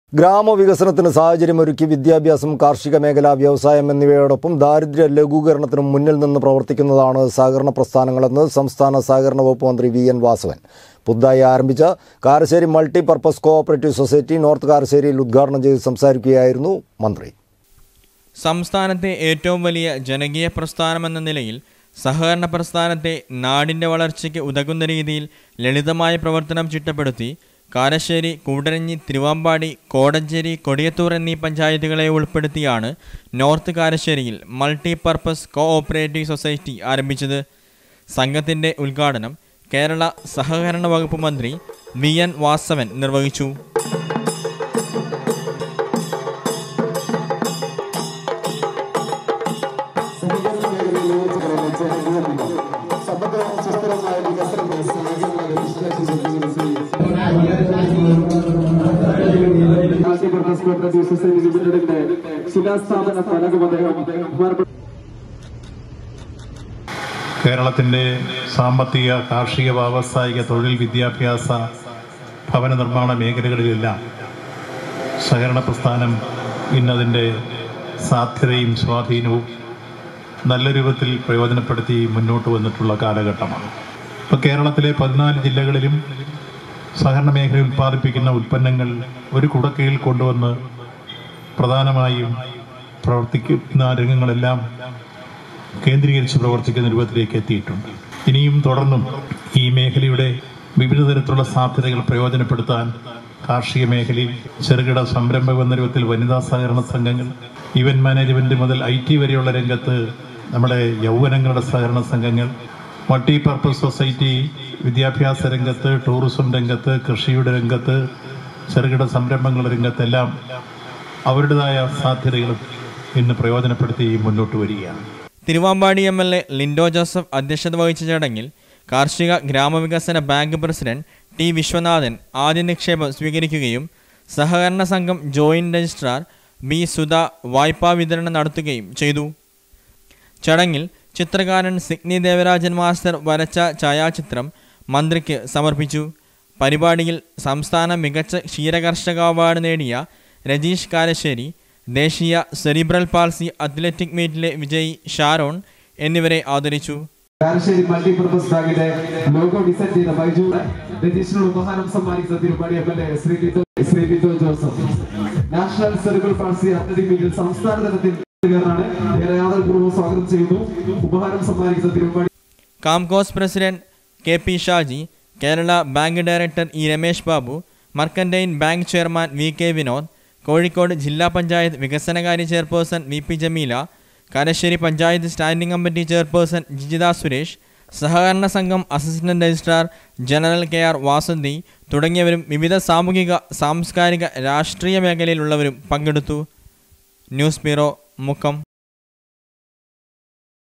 ഗ്രാമ വികസനത്തിന്‌ സാഹചര്യം ഒരുക്കി വിദ്യാഭ്യാസം, കാർഷിക മേഖല, വ്യവസായ എന്നിവയോടൊപ്പം
ഗ്രാമ വികസനത്തിന്‌ സാഹചര്യം ഒരുക്കി വിദ്യാഭ്യാസം, കാർഷിക മേഖല, വ്യവസായ എന്നിവയോടൊപ്പം ദാരിദ്ര്യ ലഘൂകരണത്തിനും  മുന്നിൽ നിന്ന് പ്രവർത്തിക്കുന്നതാണ് സഹകരണ പ്രസ്ഥാനങ്ങൾ എന്ന് സംസ്ഥാന സഹകരണ വകുപ്പ് മന്ത്രി വി. എൻ വാസവൻ